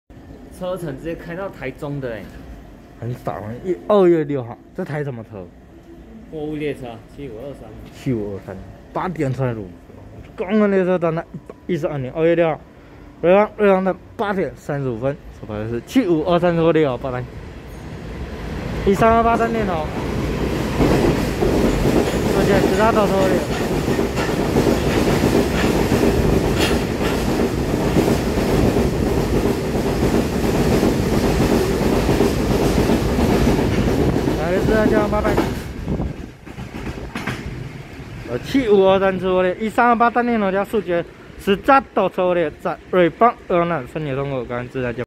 7523次E328單電頭+數節石渣斗車貨列
112/2/6瑞芳站8:35分拍攝7523次E328單電頭+數節石渣斗車貨列